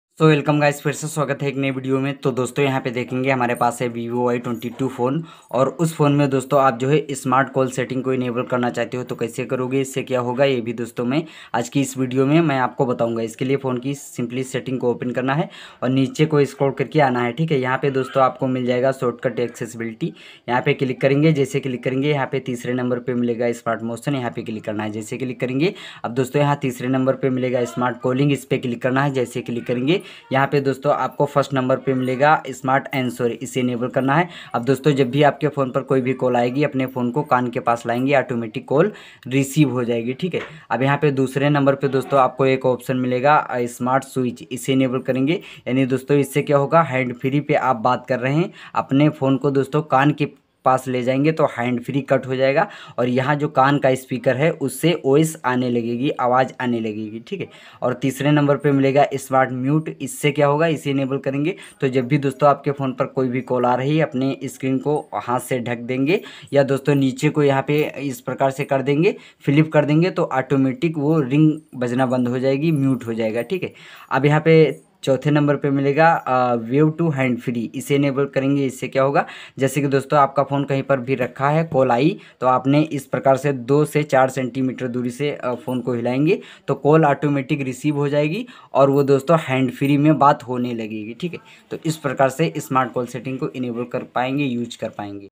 Vivo y22 smart call setting / how to enable smart call Vivo y22 / smart call feature Vivo y22
Hello dosto aaj ki is video mein humne aapko bataya Vivo y22 mobile me smart call setting ko enable kaise kiya jata hai